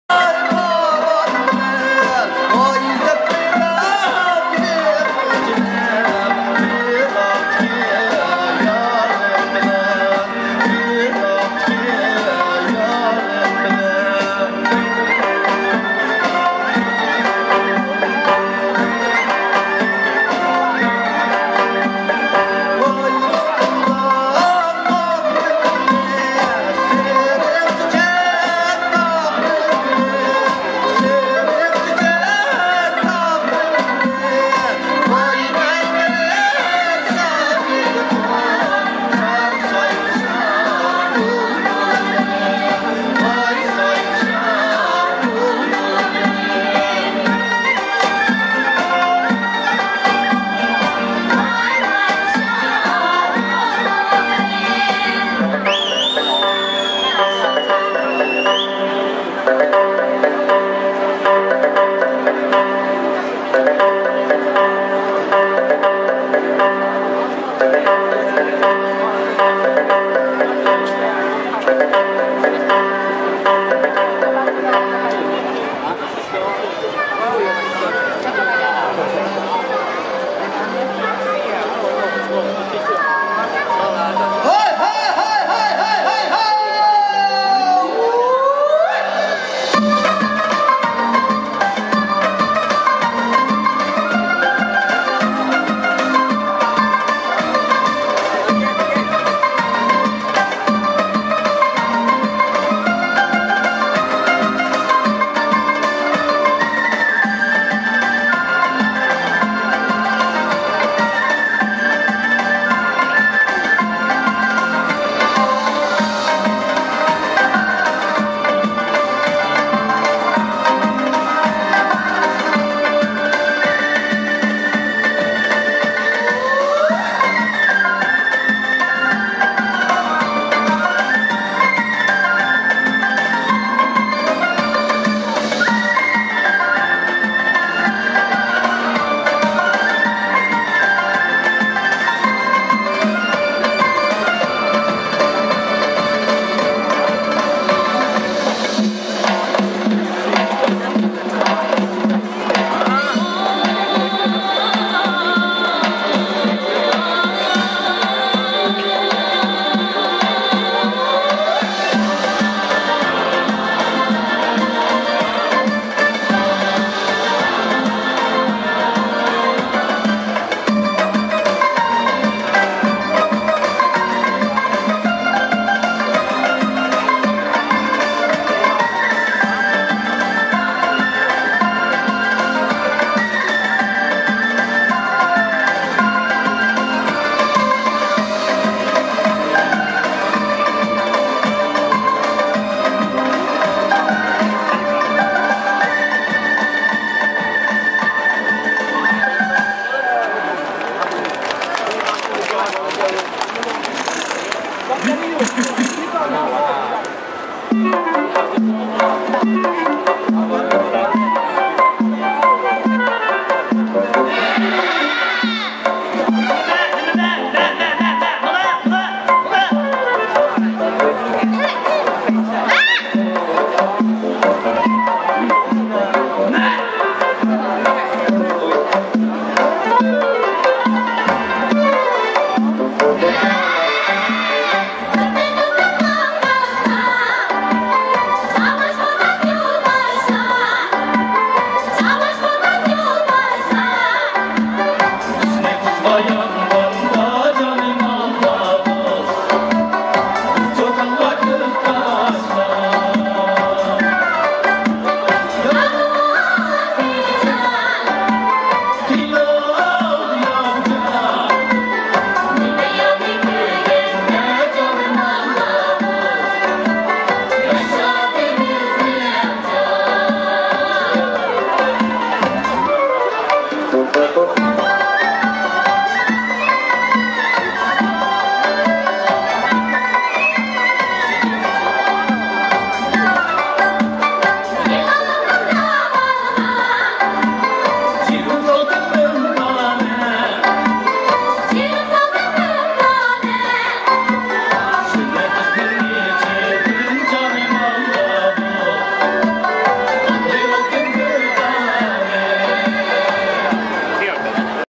River HongBao 春到河畔 2015: Welcoming the Year of The Goat MAH03513
A dance performance by members of the MuQam Troupe of XinJiang Normal University 新疆师范大学 to welcome the Year of the Goat.

Part of the Lunar New Year Celebration activities held along the eastern bank of Marina Bay, Singapore.